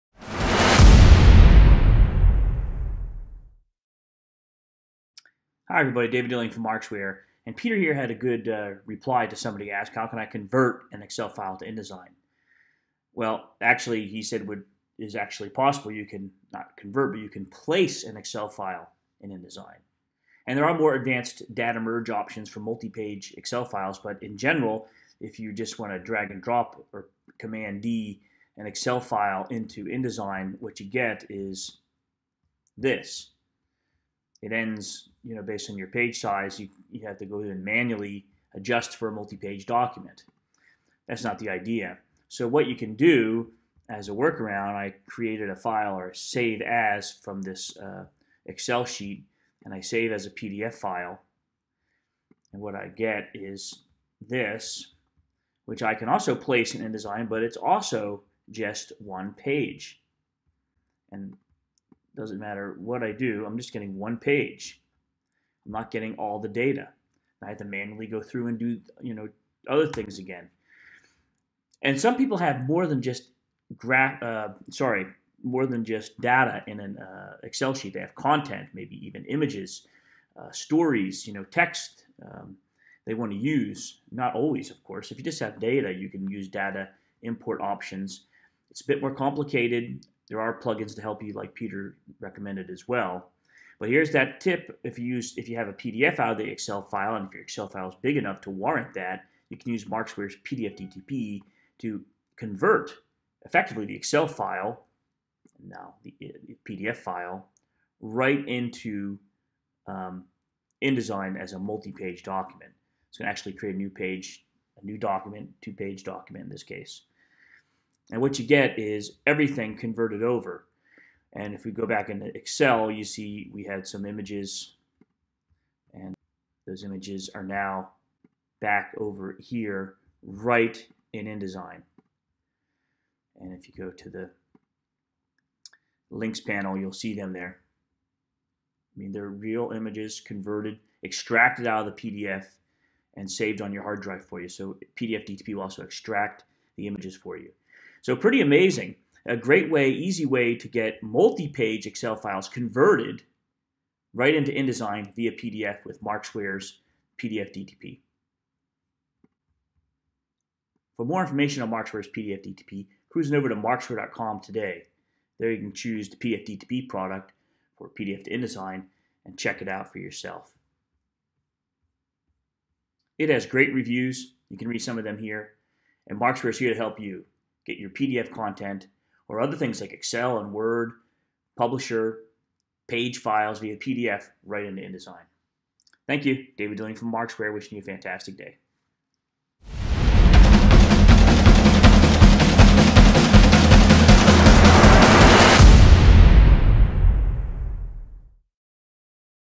How to Convert Excel to InDesign - Multi page documents
How to convert Excel to InDesign, even multi page documents: Convert an Excel file to InDesign, not only as a single, placed page. Even Adobe InDesign allows that. No actually converting all tables, text, styles, colors and images or graphics into a new InDesign file. Convert multi-page Excel files to InDesign!

How to Convert Excel to InDesign:
1) Save .XLS Excel file as a .PDF (Portable Document Format)
2) Get Markzware PDF2DTP [PDFMarkz replaces PDF2DTP]
3) Convert PDF to InDesign, with a click
4) Now you have a new, multi-page .INDD InDesign file from your Excel content

More than just the numbers or data conversion, but full content with Markzware's solution. XLS to INDD or XLS to InDesign with a click!

Note: Markzware's stand-alone PDFMarkz application replaces the PDF2DTP InDesign plug-in and allows users to open PDF files in InDesign, Affinity Publisher, and other formats: